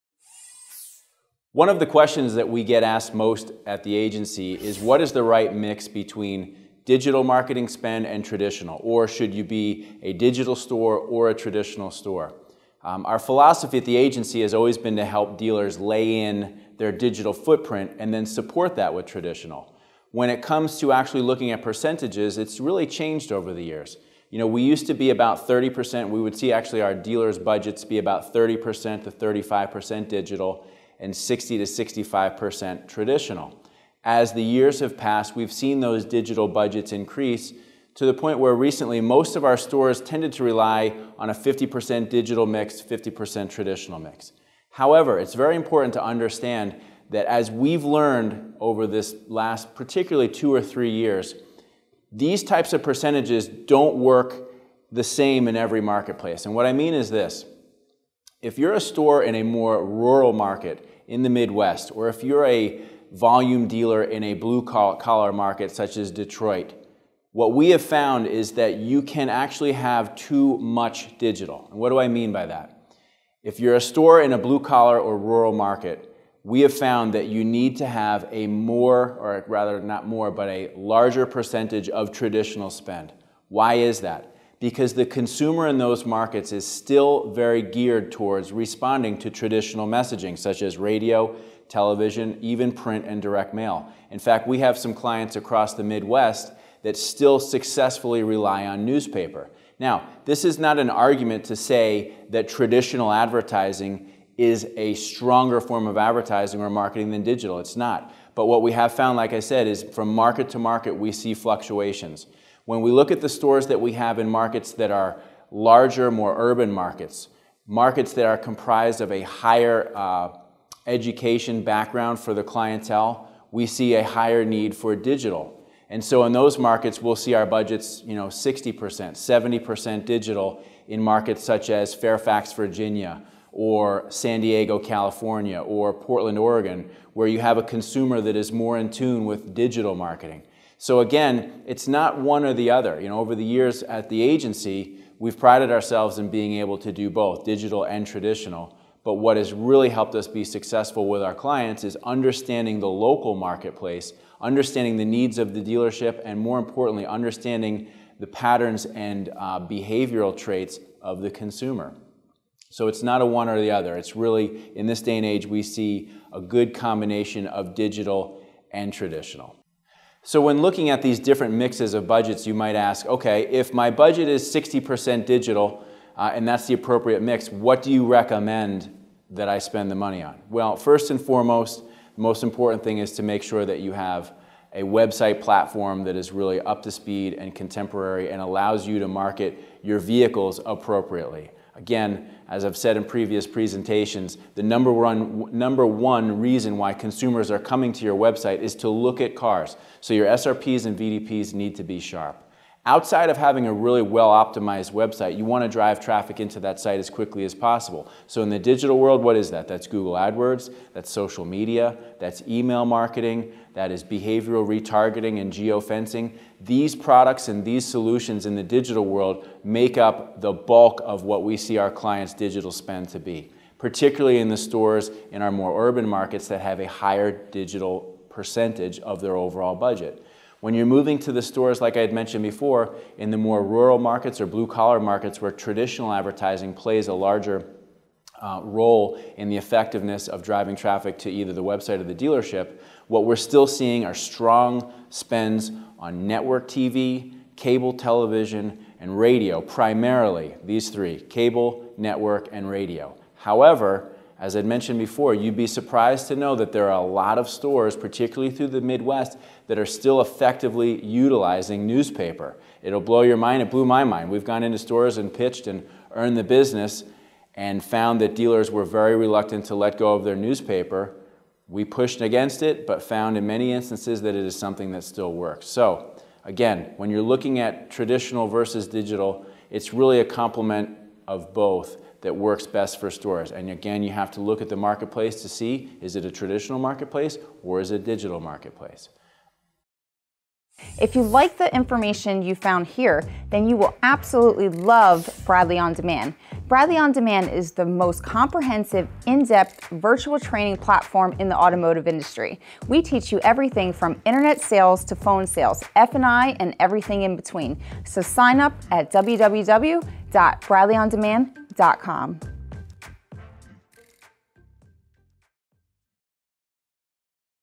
Herman Advertising Creates Balance Between Digital and Conventional Marketing
Dealer Synergy Presents Chris Herman, President of Herman Advertising, advising dealerships on balancing there marketing plans. While many ask the question whether dealerships should use conventional or digital marketing, Chris states every dealership has a different balance between the two.

If you are interested in working with Dealer Video Production,